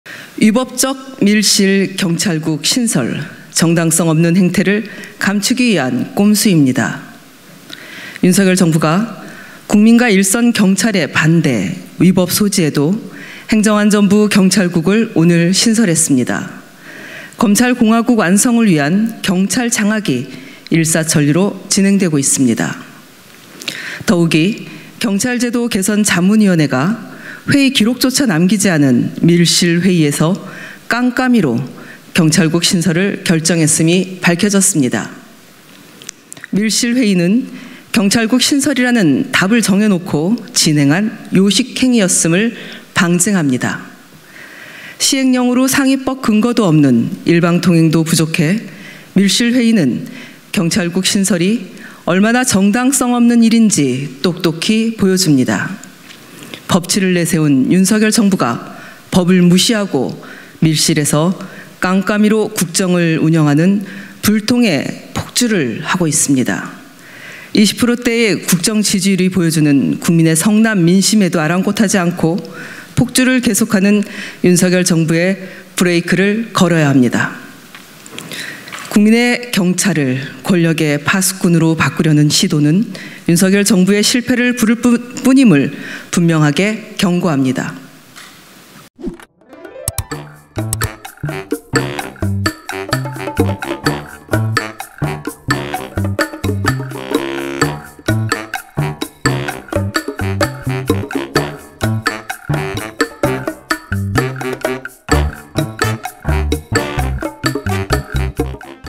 행안부 경찰국 신설에 민주당 "검찰공화국 완성 위한 경찰 장악이야"
더불어민주당 이수진 원내대변인이 22년 8월 2일 국회 소통관 기자회견장에서 행정안전부의 경찰국 신설과 관련해 기자회견을 하고 있다.